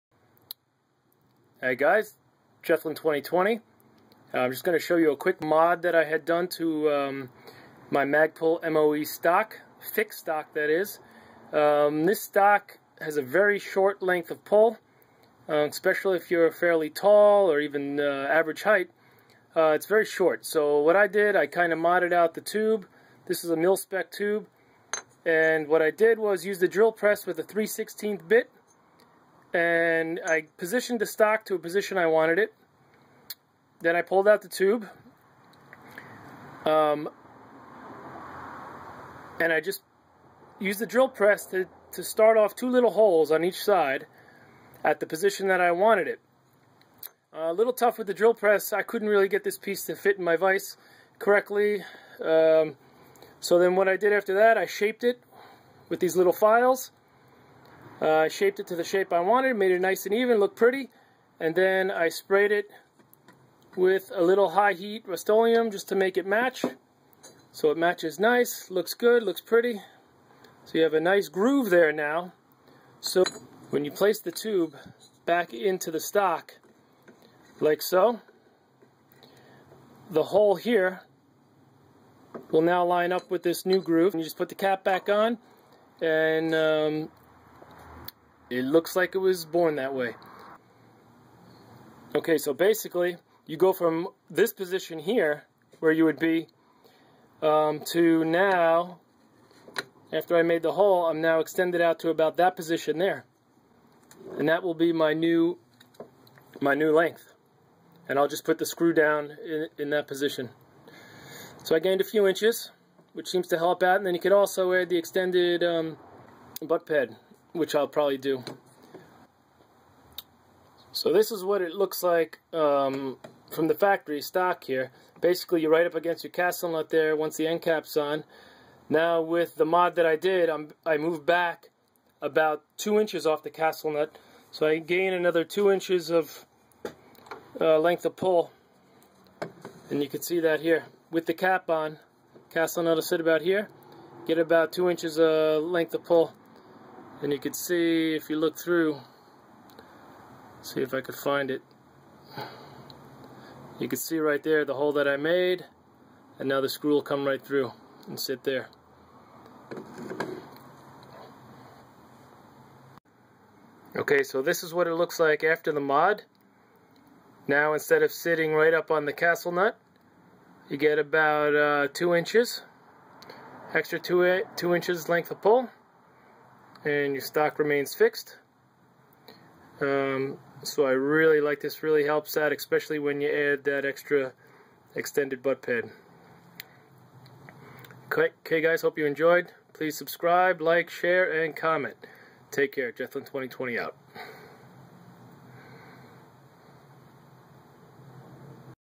HOW TO: Magpul MOE Stock Length Of Pull Mod
In this video I show you how I modified my milspec buffer tube to increase length of pull with the Magpul MOE stock.